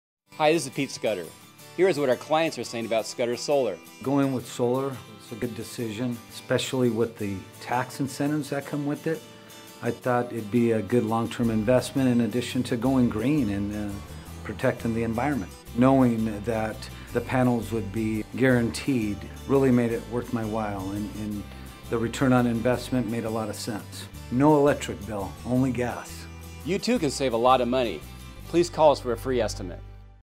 Scudder solar - Rich Aiello Testimonial
Hear what Scudder Solar customer Rich Aiello has to say about working with us!